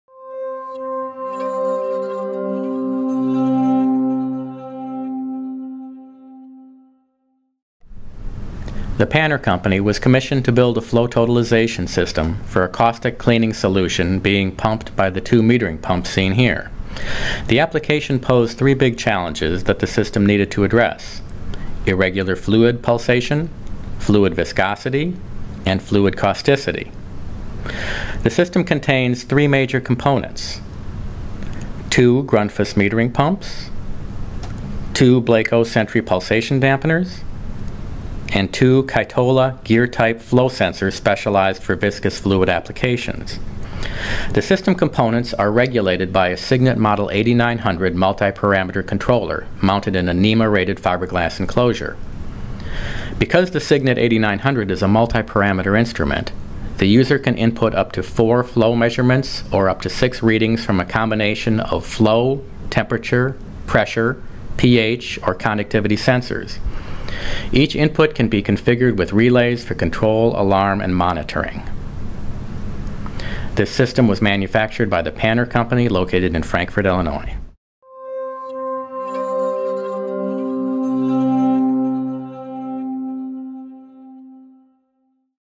Dual-flow measurement system using Signet multi-parameter controller
This is a flow measurement system from the Panner Company which is used to measure a viscous, caustic cleaning solution. The system uses a Signet model-8900 multi-parameter controller to accept two inputs. Because of pulsating flow, the system must use a Blacoh pulsation dampener prior to measurement by the flow sensors. This system is used to measure the total amount of cleaning solution used over time, and may be expanded in the future to include up to four additional input sensors for parameters such as pH/ORP, temperature, pressure, or conductivity.